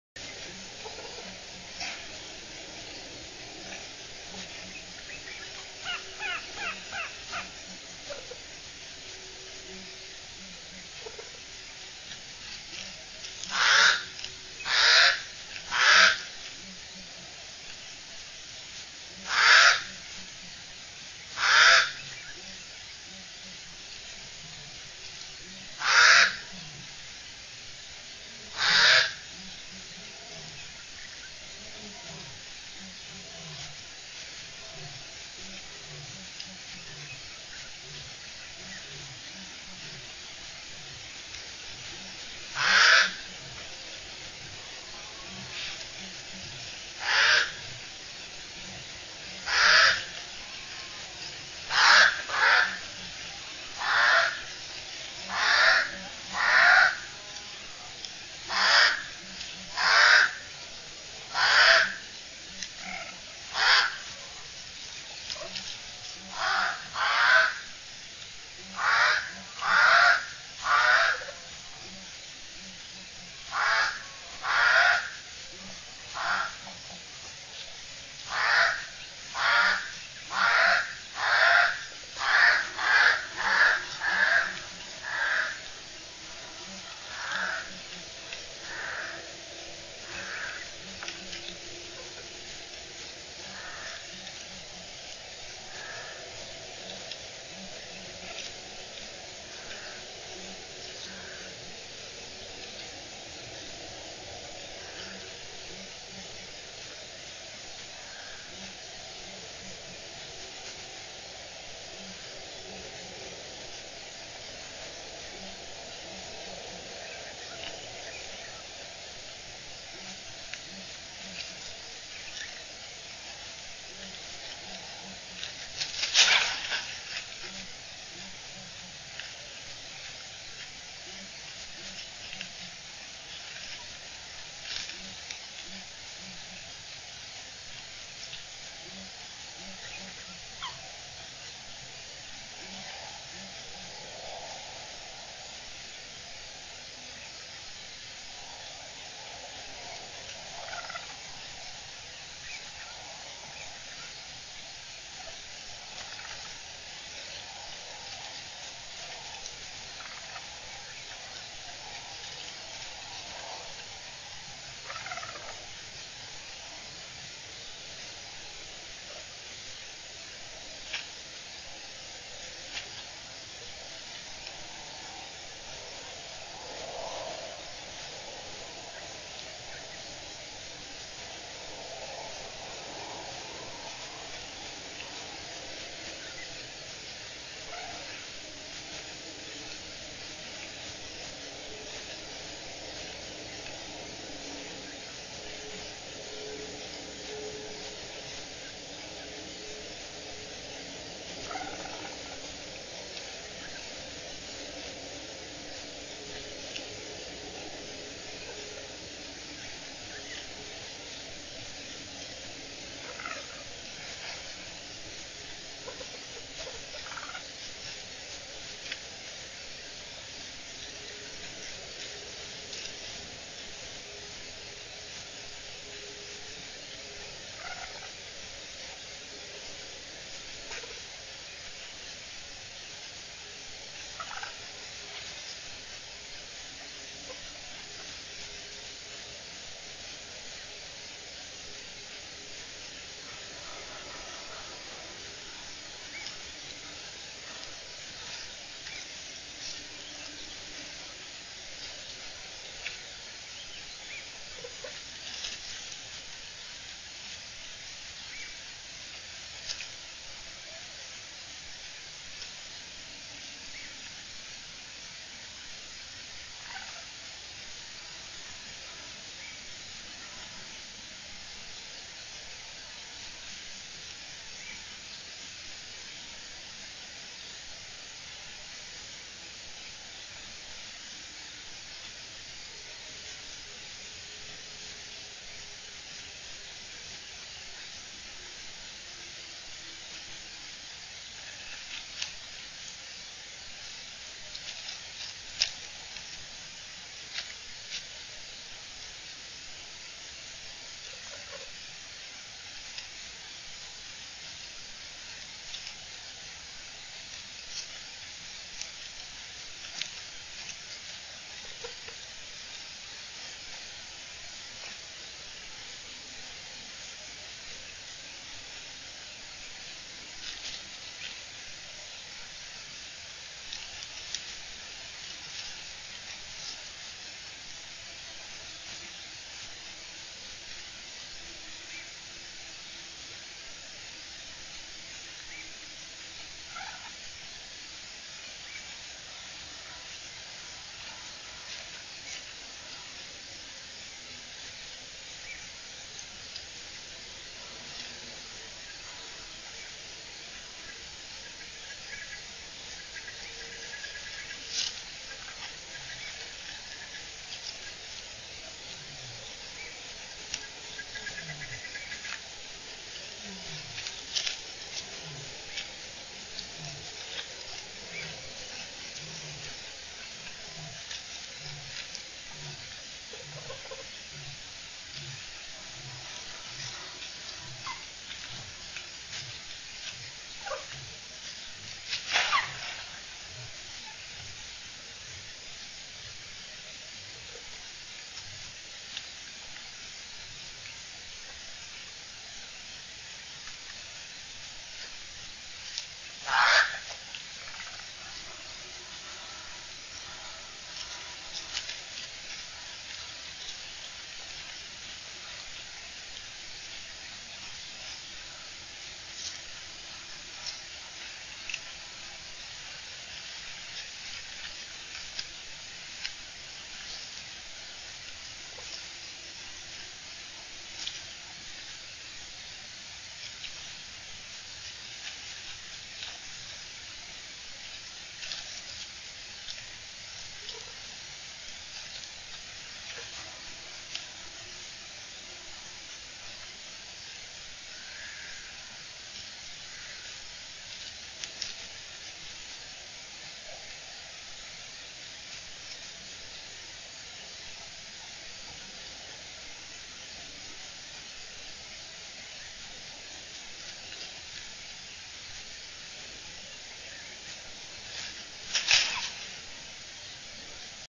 Cheetahs at Hoedspruit Endangered Species Center, June 10, 2012
All three of Cheetah Leah's cubs crawl under the fence to go play with Salome's three cubs.